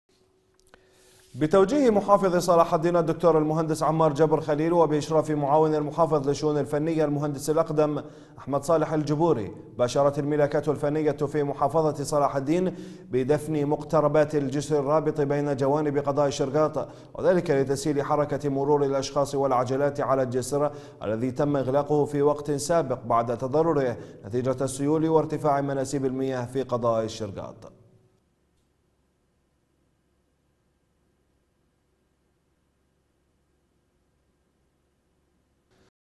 توجيه محافظ صلاح الدين الملاكات الفنية في المحافظة تباشر بدفن مقتربات جس الشرقاط
توجيه محافظ صلاح الدين
الملاكات الفنية في المحافظة تباشر بدفن مقتربات جس الشرقاط

بتوجيه محافظ صلاح الدين الدكتور المهندس عمار جبر خليل وباشراف معاون المحافظ للشؤون الفنية المهندس الاقدم احمد صالح الجبوري باشرت الملاكات الفنية في محافظة صلاح الدين بدفن مقتربات الجسر الرابط بين جوانب قضاء الشرقاط , وذلك لتسهيل حركة مرور الاشخاص والعجلات على الجسر الذي تم اغلاقه في وقت سابق بعد تضرره نتيجة السيول وارتفاع مناسيب المياه في قضاء الشرقاط  .